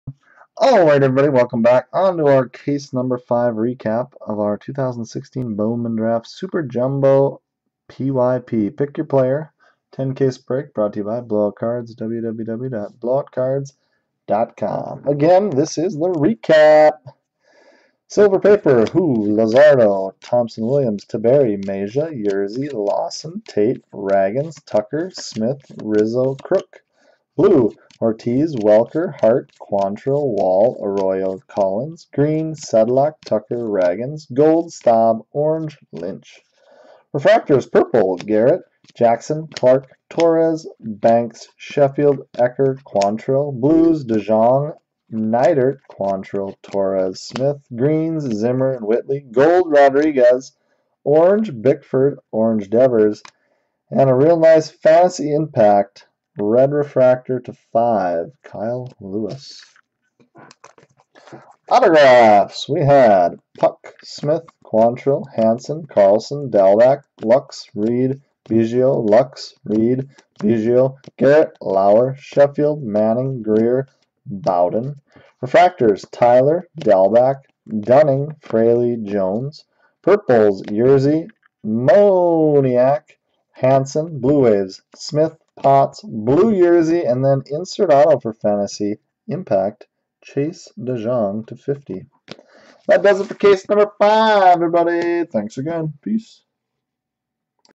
2016 Bowman Draft SUPER JUMBO 10 Case Player Break Blowout Cards 2-10-17 Case #5 Recap
2016 Bowman Draft SUPER JUMBO 10 Case Player Break Blowout Cards 2-10-17 Case #5 Recap - Captured Live on Ustream